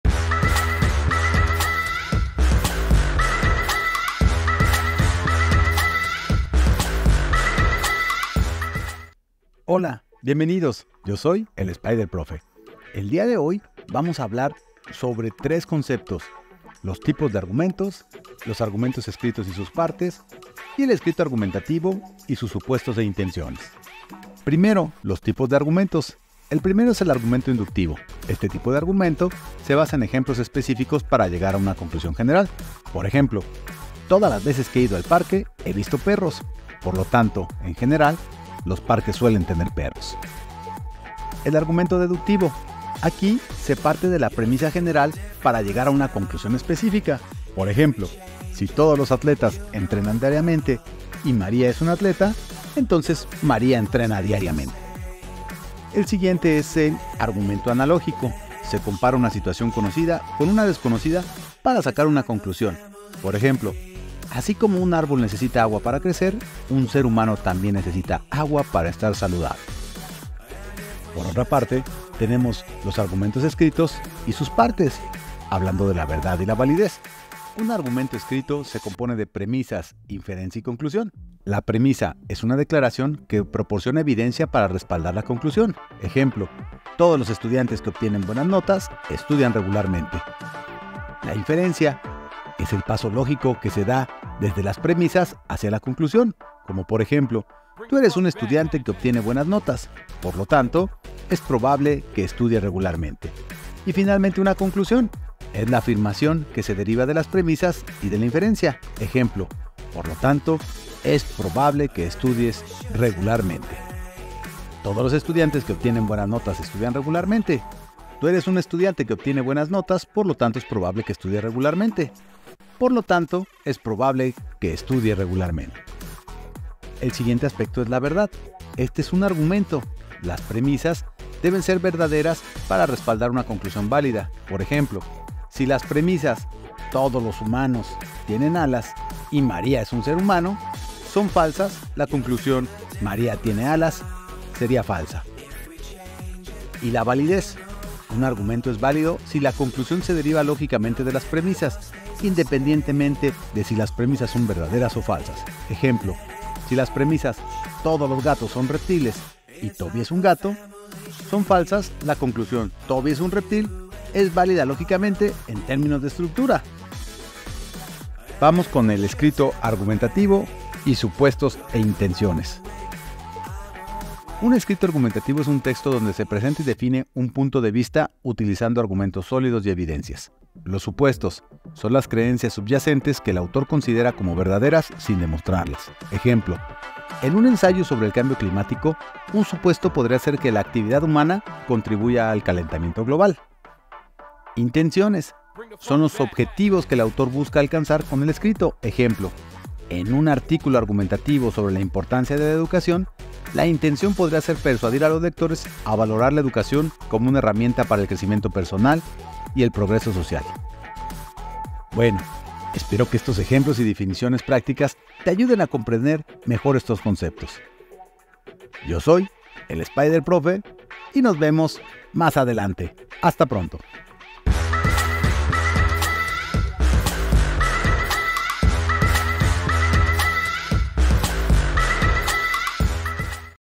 Spider Profe - Tipos de Argumentos - Progresión.10 - Humanidades Dim. 1 - 3
1. **Tipos de Argumentos**:

   - **Argumento Inductivo**: Este tipo de argumento se basa en ejemplos específicos para llegar a una conclusión general. Ejemplo: "Todas las veces que he ido al parque, he visto perros. Por lo tanto, en general, los parques suelen tener perros."

   - **Argumento Deductivo**: Aquí, se parte de premisas generales para llegar a una conclusión específica. Ejemplo: "Si todos los atletas entrenan diariamente y María es una atleta, entonces María entrena diariamente."

   - **Argumento Analógico**: Se compara una situación conocida con una desconocida para sacar una conclusión. Ejemplo: "Así como un árbol necesita agua para crecer, un ser humano también necesita agua para estar saludable."

2. **Argumento Escrito y sus Partes (Verdad/Validez)**:

   Un argumento escrito se compone de premisas, inferencia y conclusión.

   - **Premisa**: Es una declaración que proporciona evidencia para respaldar la conclusión. Ejemplo: "Todos los estudiantes que obtienen buenas notas estudian regularmente."

   - **Inferencia**: Es el paso lógico que se da desde las premisas hacia la conclusión. Ejemplo: "Tú eres un estudiante que obtiene buenas notas, por lo tanto, es probable que estudies regularmente."

   - **Conclusión**: Es la afirmación que se deriva de las premisas e inferencia. Ejemplo: "Por lo tanto, es probable que estudies regularmente."

   **Verdad**: En un argumento, las premisas deben ser verdaderas para respaldar una conclusión válida. Ejemplo: Si las premisas "Todos los seres humanos tienen alas" y "María es un ser humano" son falsas, la conclusión "María tiene alas" sería falsa.

   **Validez**: Un argumento es válido si la conclusión se deriva lógicamente de las premisas, independientemente de si las premisas son verdaderas o falsas. Ejemplo: Si las premisas "Todos los gatos son reptiles" y "Toby es un gato" son falsas, aún la conclusión "Toby es un reptil" es válida lógicamente en términos de estructura.

3. **Escrito Argumentativo y Supuestos e Intenciones**:

   Un escrito argumentativo es un texto donde se presenta y defiende un punto de vista utilizando argumentos sólidos y evidencia.

   - **Supuestos**: Son las creencias subyacentes que el autor considera como verdaderas sin demostrarlas. Ejemplo: En un ensayo sobre el cambio climático, un supuesto podría ser que la actividad humana contribuye al calentamiento global.

   - **Intenciones**: Son los objetivos que el autor busca alcanzar con el escrito. Ejemplo: En un artículo argumentativo sobre la importancia de la educación, la intención podría ser persuadir a los lectores a valorar la educación como una herramienta para el crecimiento personal y el progreso social.

Espero que estos ejemplos y definiciones prácticas te ayuden a comprender mejor estos conceptos. Si tienes más preguntas o necesitas más aclaraciones, no dudes en preguntar.